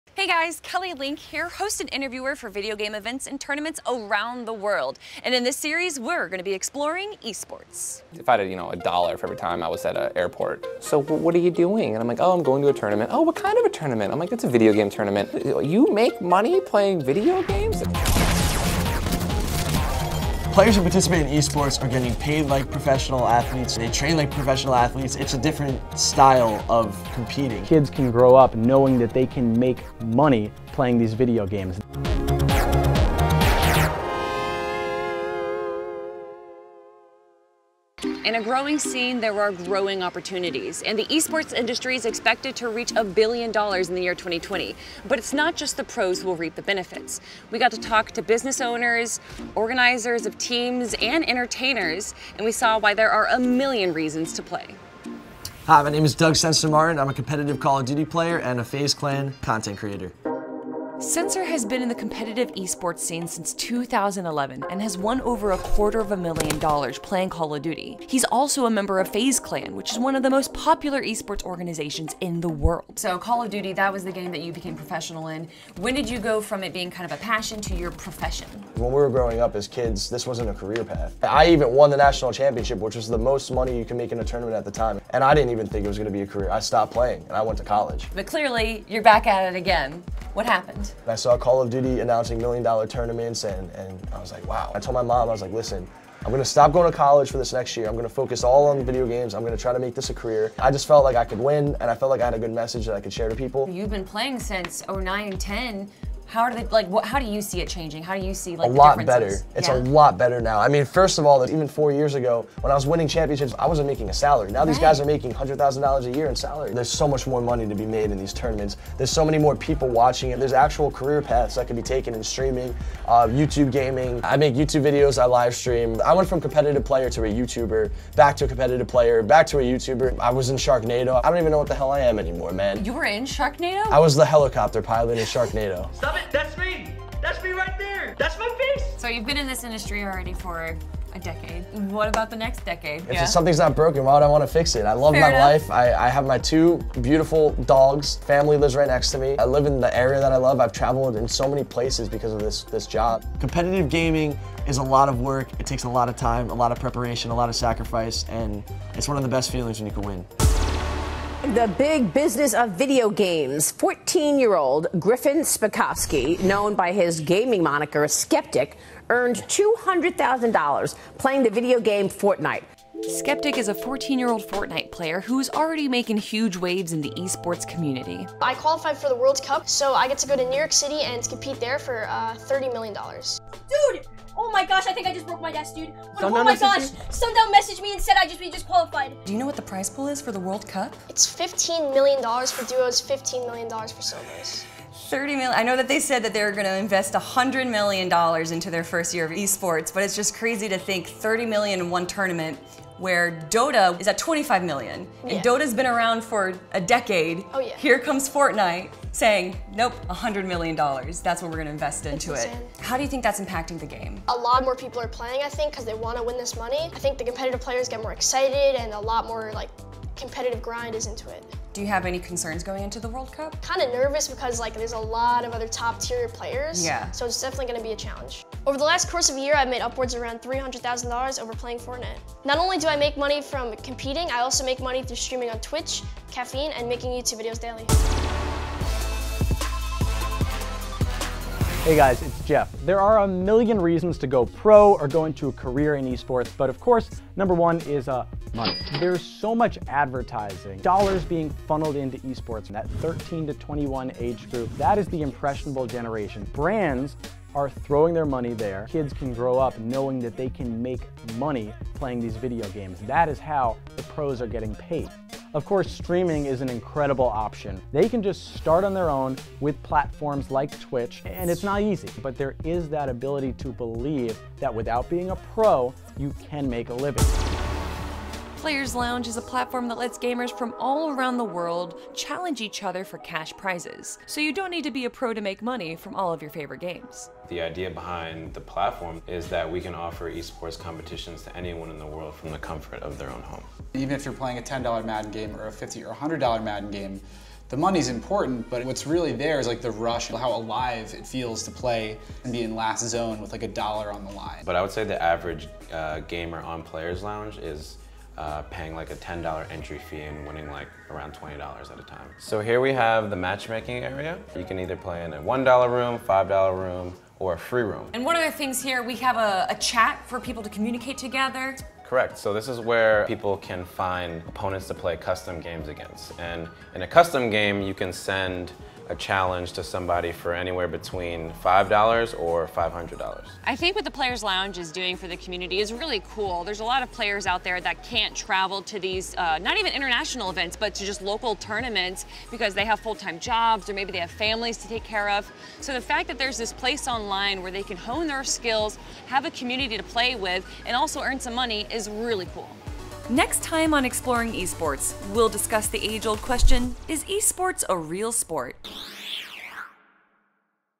How Gamers Make MILLIONS From Esports | FaZe Censor & Sceptic
Gamers are making BANK off of their talents! Learn how from FaZe Censor & Sceptic.

Exploring Esports is our new series gives you a never before seen look into the world of GAMING!  COMMENT with what gamer you want to see us feature next!

The music we use - AMAZING for YouTubers!